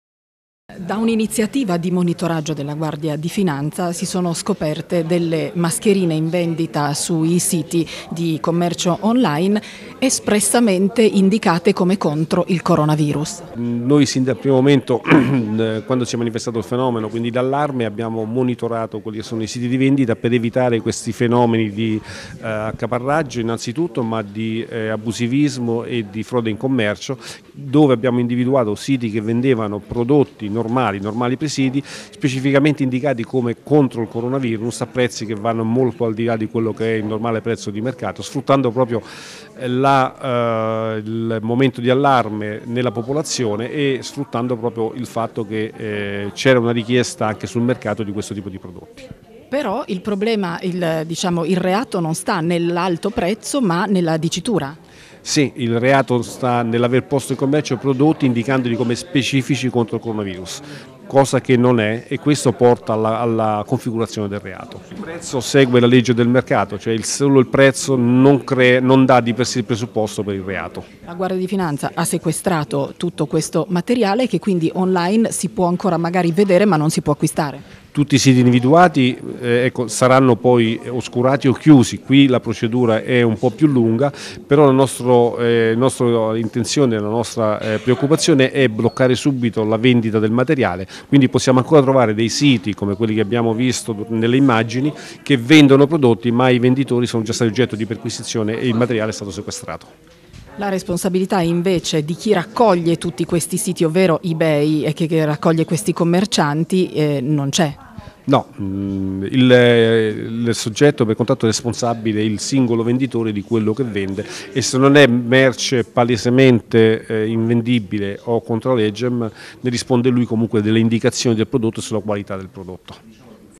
MASCHERINE PER CORONAVIRUS A 5 MILA EURO, 22 IMPRENDITORI INDAGATI
Mascherine vendute anche a cinquemila euro e integratori spacciati come "protezione" dal virus. Piu' di venti gli imprenditori indagati